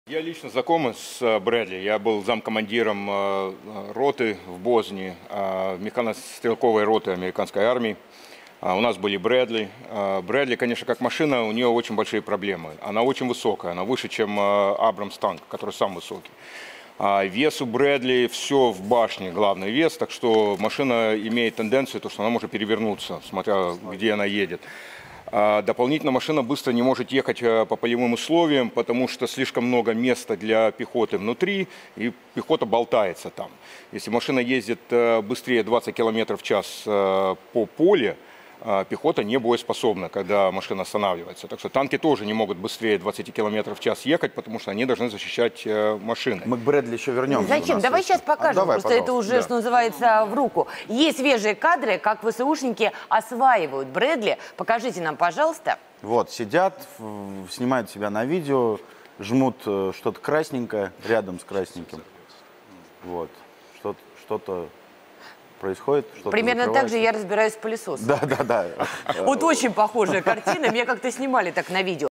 On Russian TV, a Former American Army Soldier Talks About “Massive Issues” w/ Bradley Combat Vehicle
On Russian TV, a former American army soldier, born in Luhansk, talks about massive issues that Bradley combat vehicle has, such as "having too much space inside" and not being able to go over 20km/h 🤡
Wonder what is his opinion on the Soviet vehicles.